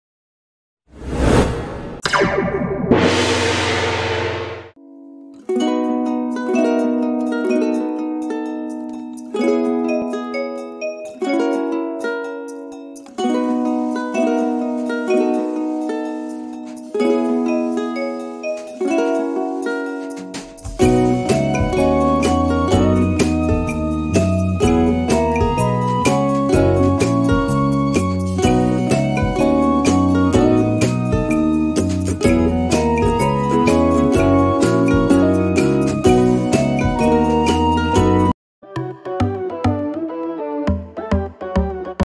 Most Beautiful Red Snail | Moving Slow and Nice - Feel Relaxing | 8K HD
Hello Guys and Welcome, Im the creator of Nature & Beauty, In this video you are going to watch the most beautiful red Snail moving. The Beauty Of Nature.

Nature & Beauty

This would be a motivation booster for us to create more relaxing videos for you..